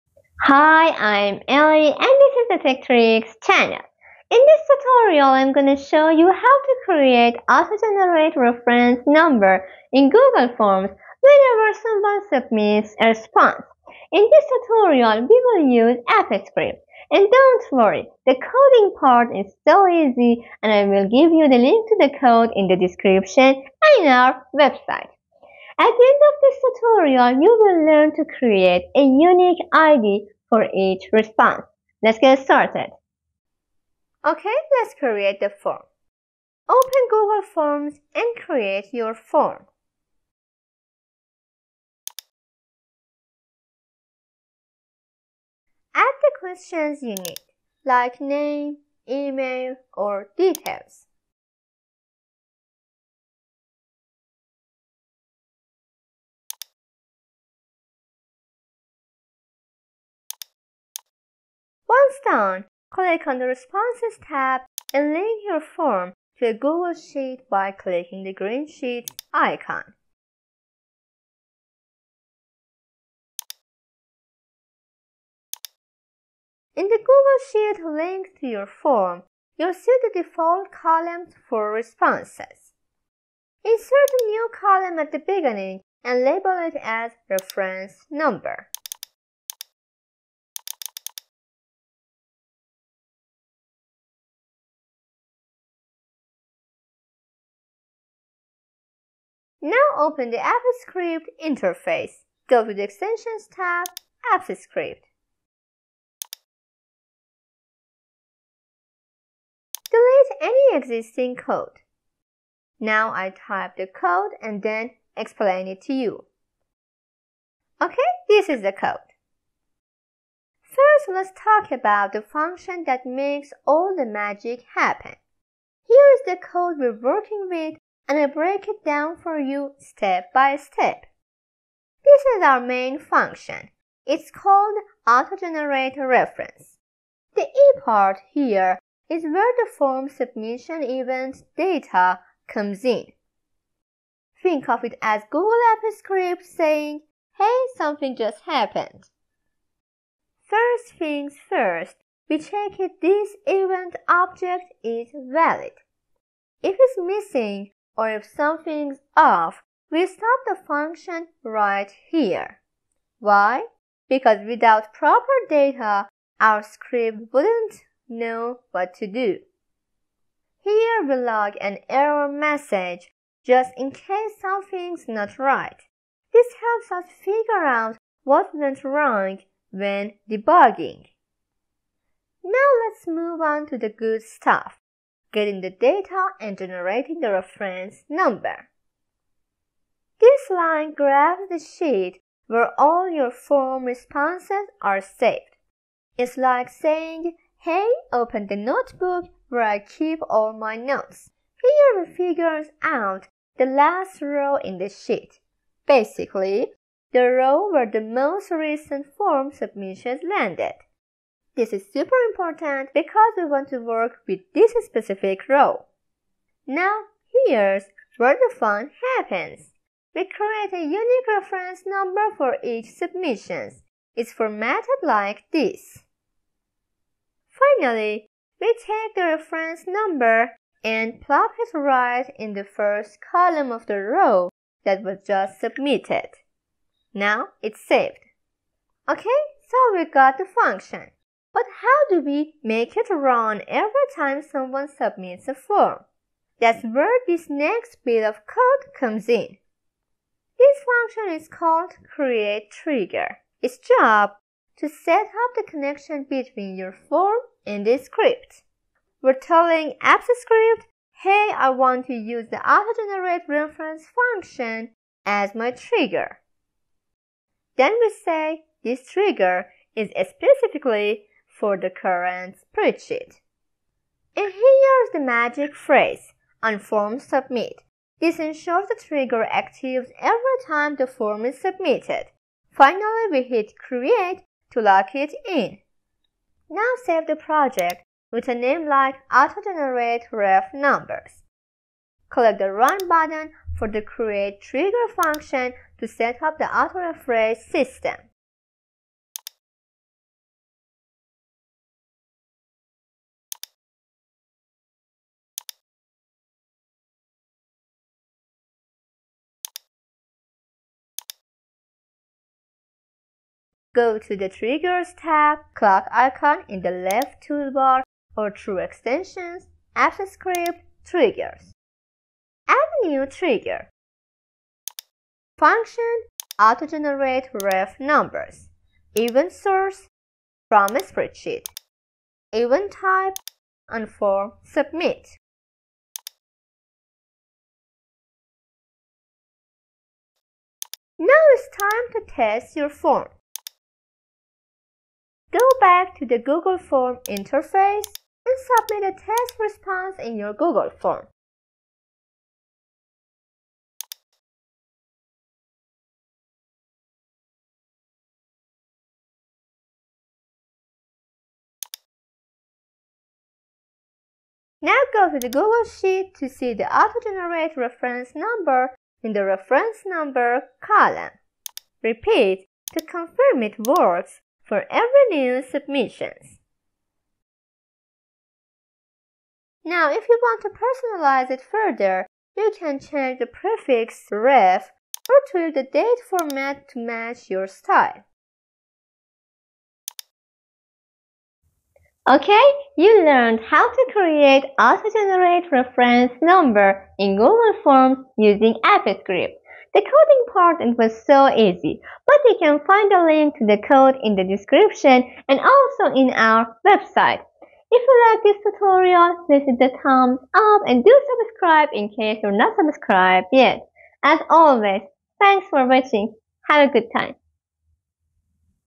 Auto Generate Reference Number When Form Submitted (Google form)
Learn how to automatically generate unique reference numbers for every Google Form submission using Google Apps Script! 🚀 Whether you're managing registrations, tracking orders, or organizing events, adding reference numbers ensures each submission is easily identifiable and professionally organized.
❤️ Link to the Form and code❤️
Timestamps
00:00 Intro
00:35 Setting up Google Forms and google sheets
01:41 Writing a simple Apps Script
05:16 Triggering the script
05:41 Test the form
06:32 Wrap up
❤️ Help me pay for my first Camera❤️